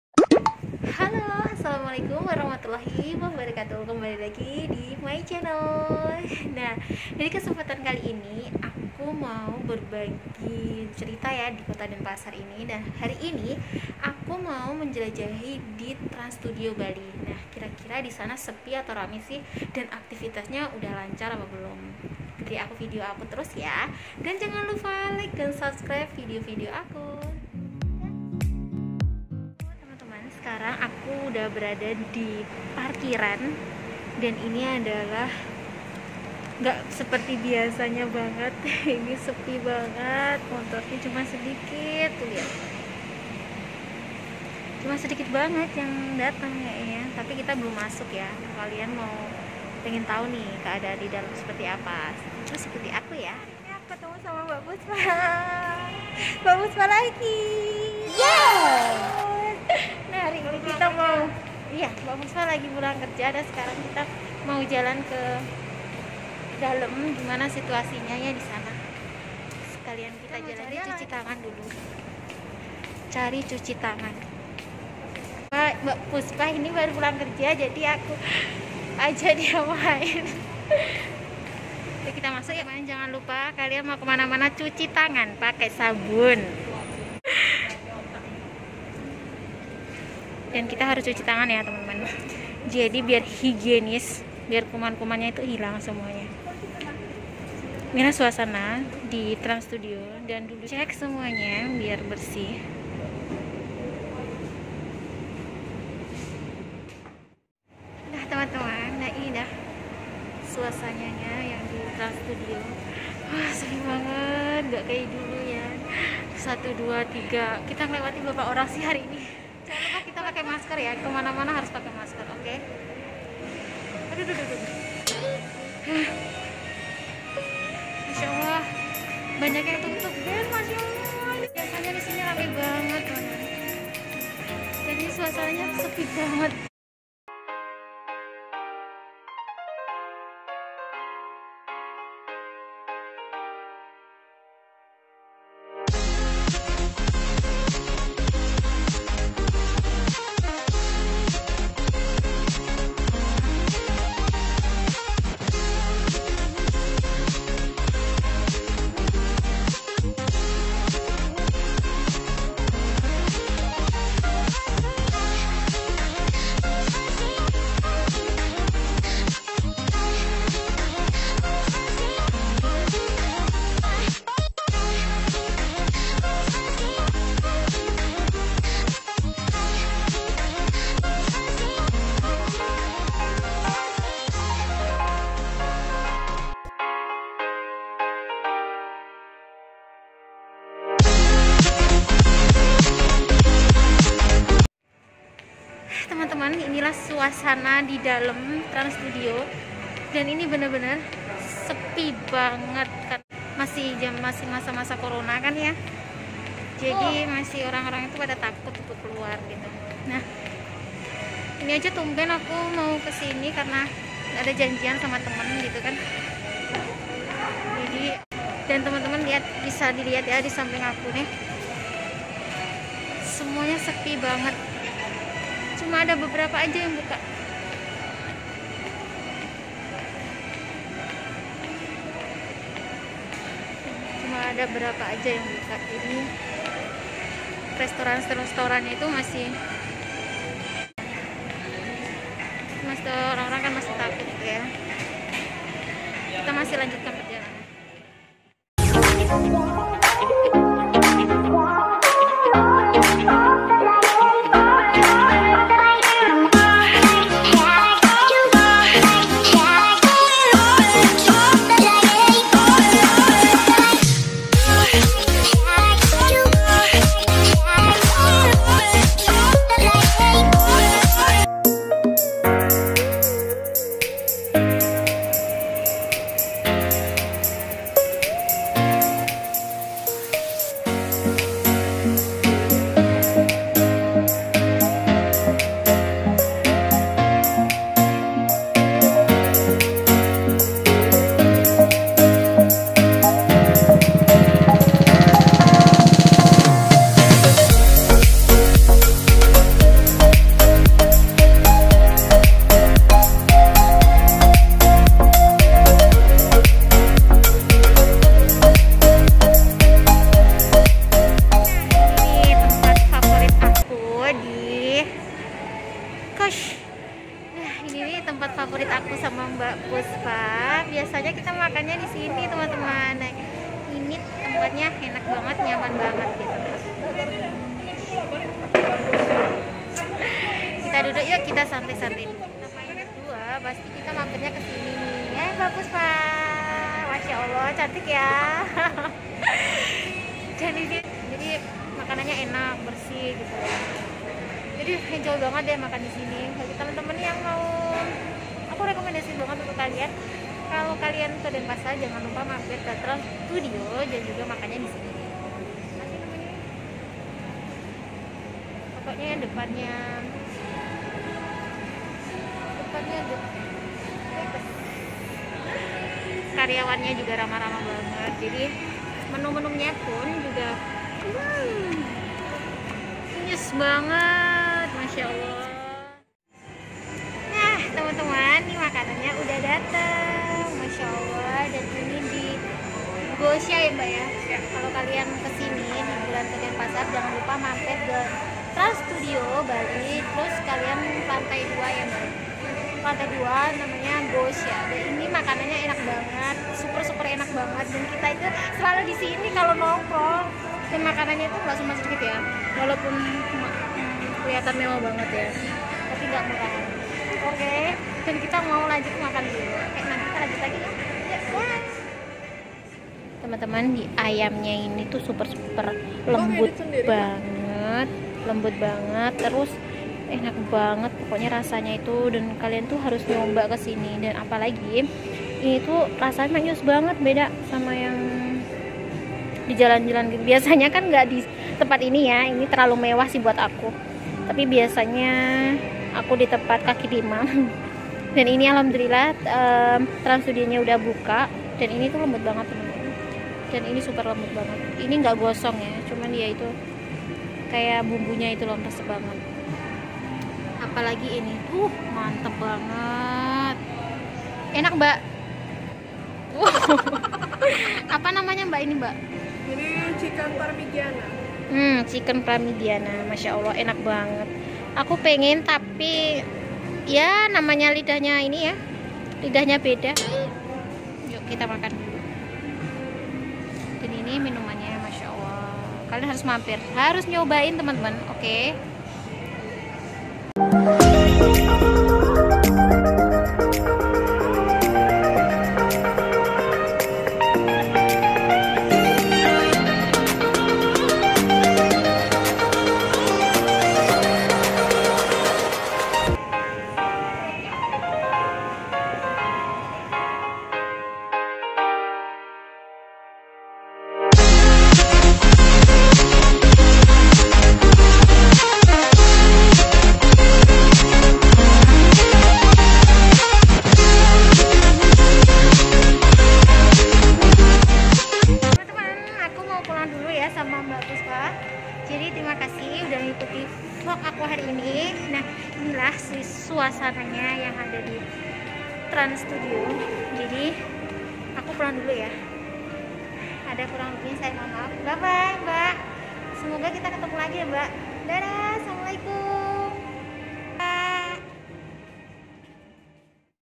TRANS STUDIO BALI||VLOG|| KETEMUAN TAMU SPESIAL
Bismillah yuk kita sama2 masuk ke stand studio Bali..dan bagaimana kondisinya skrng...

Smoga Denpasar Bali dan kota yg lain bisa kembali normal seperti semua..amin

Dan Denpasar Bali bs menjdi tempat wisata seperti dulu kala...amin